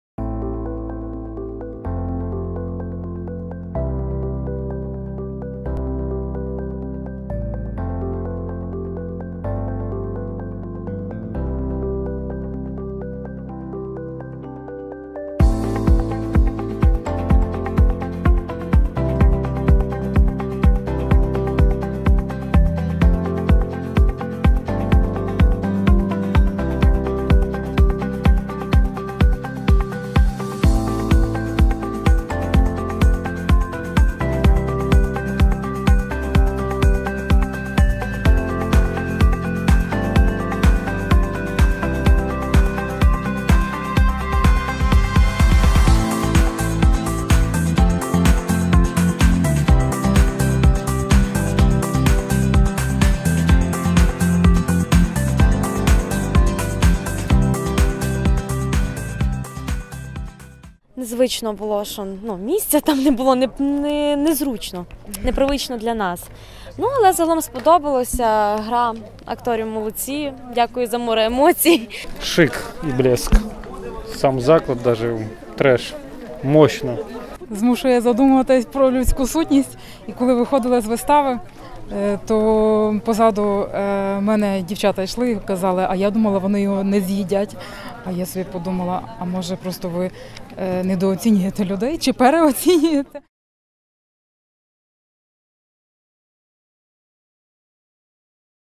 «Мандрівний вішак» | театр «ВідСутність» | «У відкритому морі»
Відеозйомка Олексія Стаднюка, монтаж Олександра Зінчука.
Відео із виступу на фестивалі театрів за межами театру «Мандрівний вішак».
Фестиваль відбувався 30 вересня - 2 жовтня 2016 року у місті Луцьк.